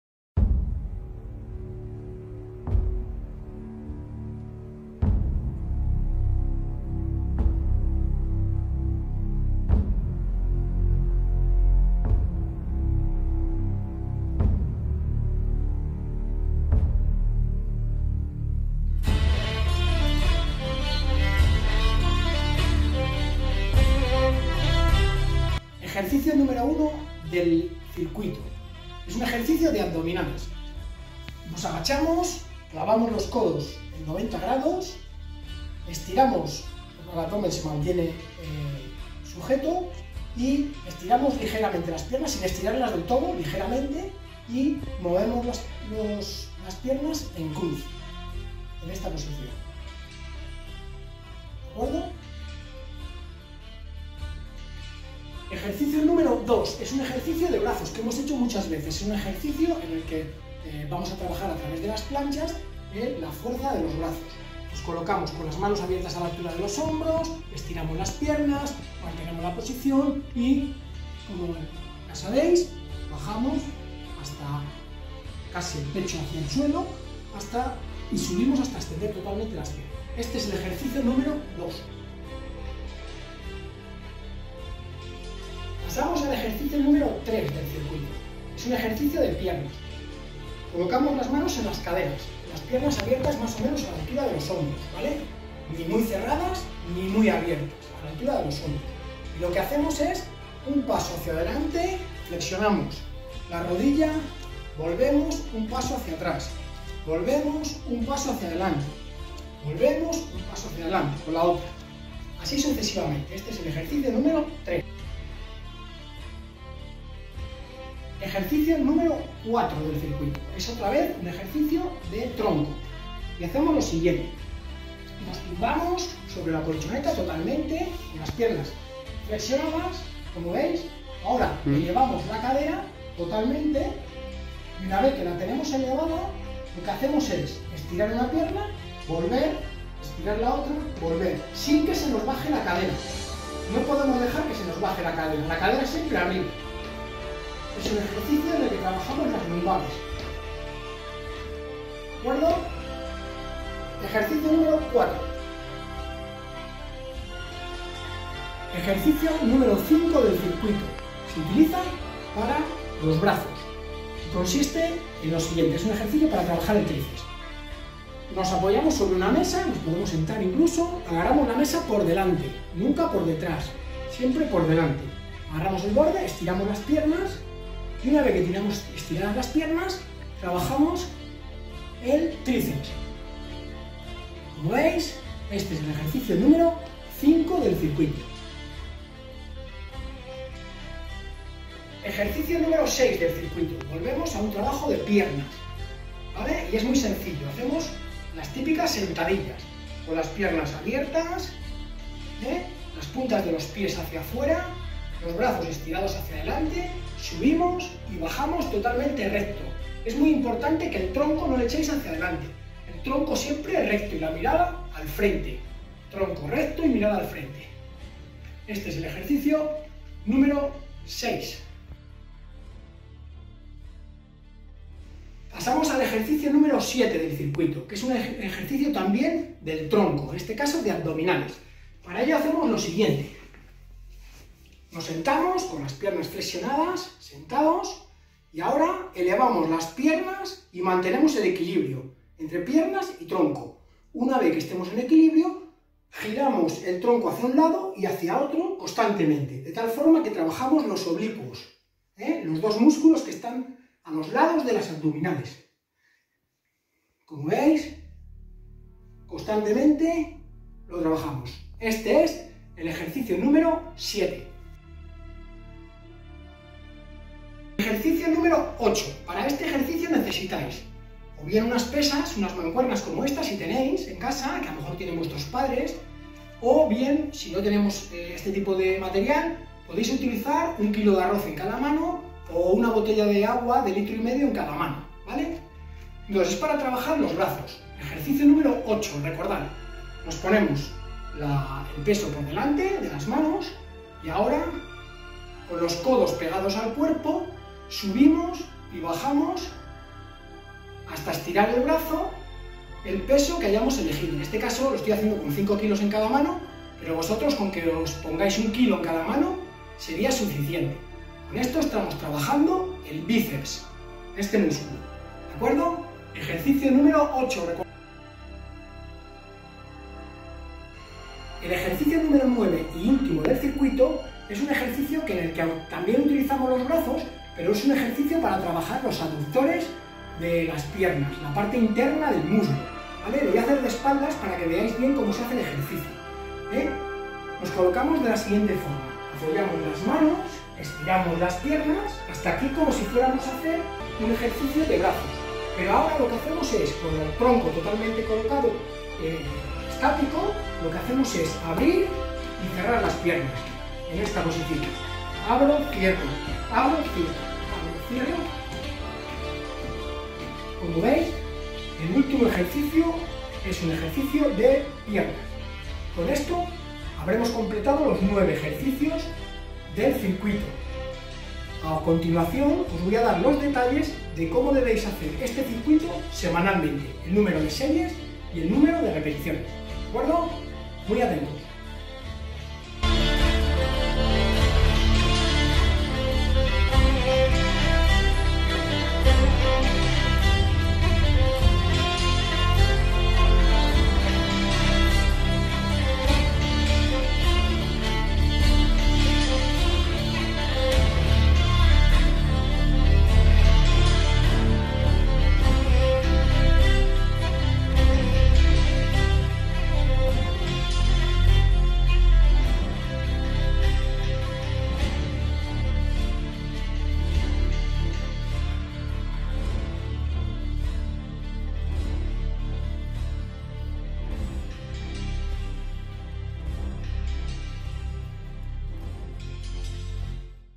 CIRCUITO FUERZA- RESISTENCIA. IES VILLALPANDO
En este vídeo obtendrás todas las pautas de trabajo para realizar correctamente el circuito en función del curso y ciclo en el que te encuentres.